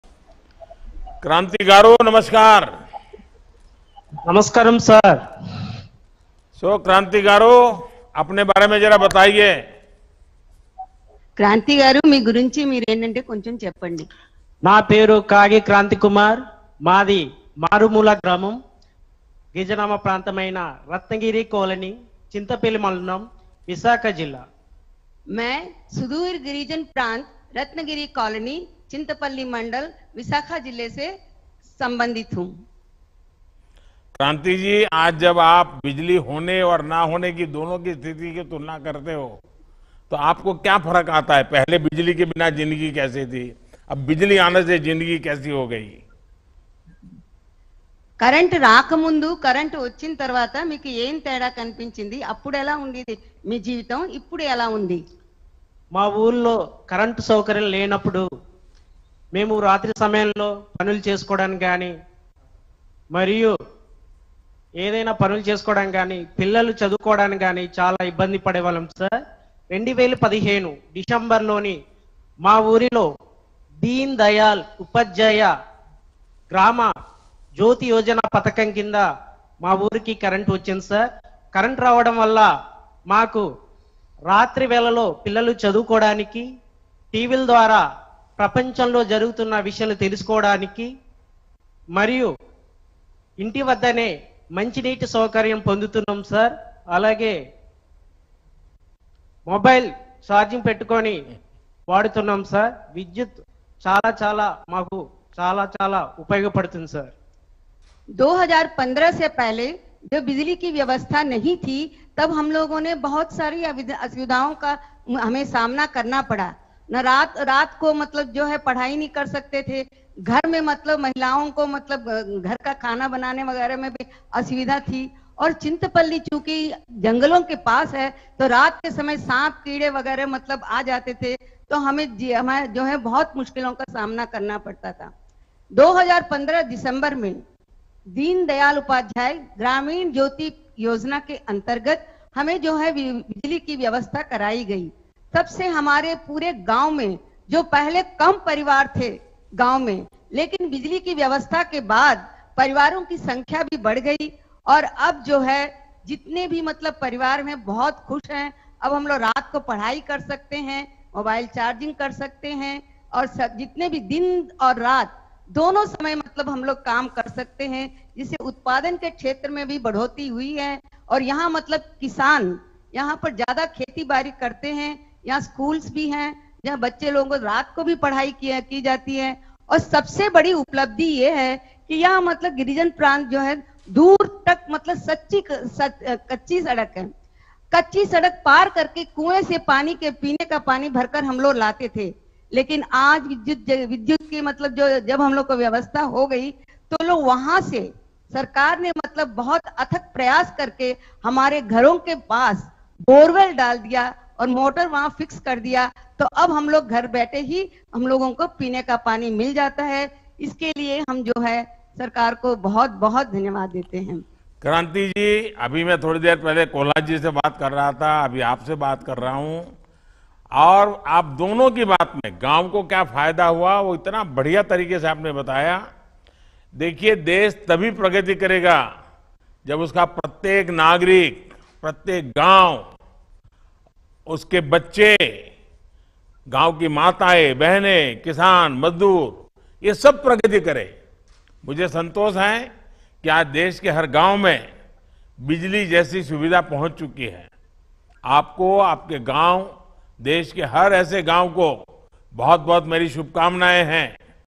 Deendayal Upadhyaya Gram Jyoti Yojana से कैसे रोशन हुआ लाभार्थी का जीवन... जानिए इस video में
PM Modi participated in the Grand Finale marking the culmination of ‘Ujjwal Bharat Ujjwal Bhavishya – Power @2047’. He launched the Revamped Distribution Sector Scheme as well as launched various green energy projects of NTPC. "Four different directions were worked together to improve the power system - Generation, Transmission, Distribution and Connection," the PM added.